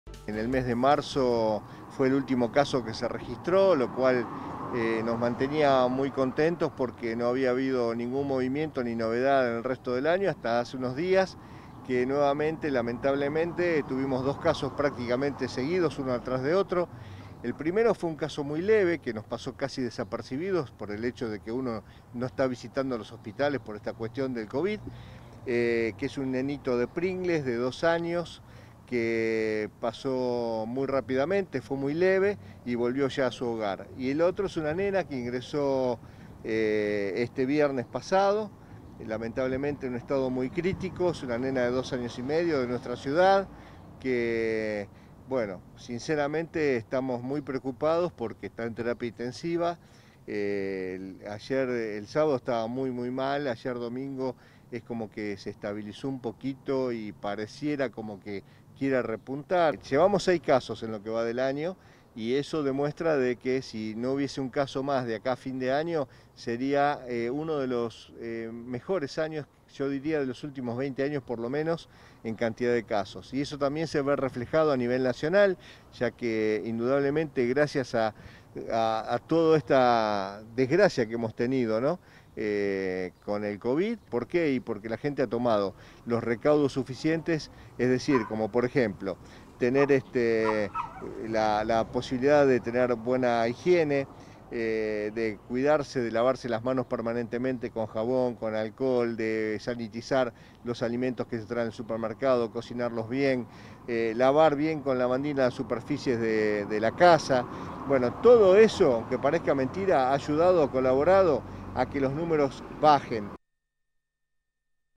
Confirman el caso de una niña de dos años que tiene SUH y está en "estado crítico"
El titular de la Asociación para la Prevención del Síndrome Urémico Hemolítico, Miguel Caracciolo, explicó que se trata del sexto caso de la enfermedad en lo que va del 2020